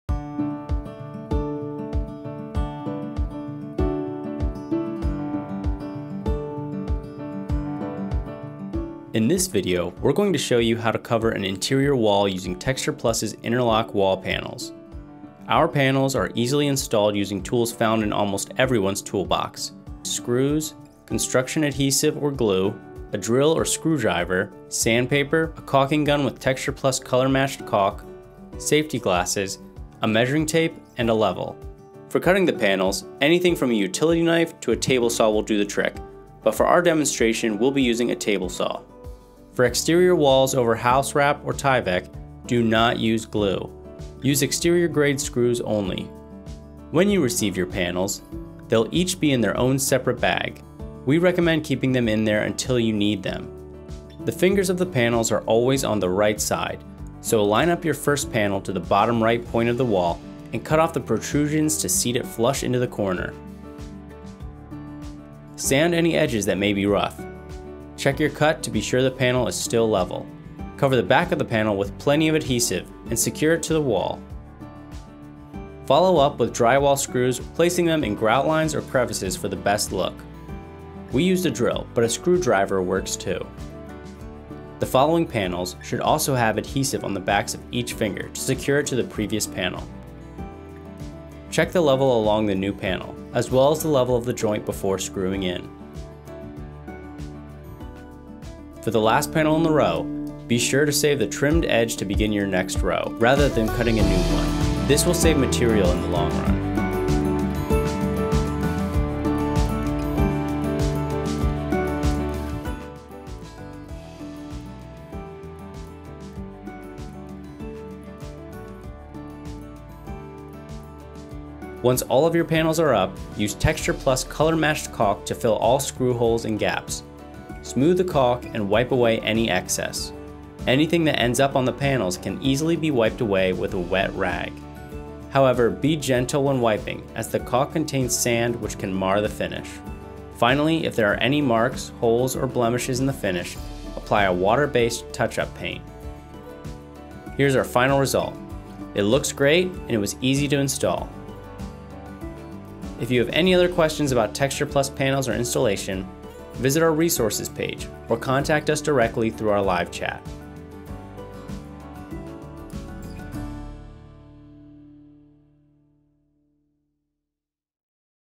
Installing Faux Wall Panels - Brick, Stone, Wood & More
Texture Plus Video Uploads
100%

In this video, we show you how to cover an interior wall using faux wall panels.

Please leave your own tips for installing faux wall panels in the comments below. Also tell us about your experience using faux wall panels for your project.
Screen reader support enabled.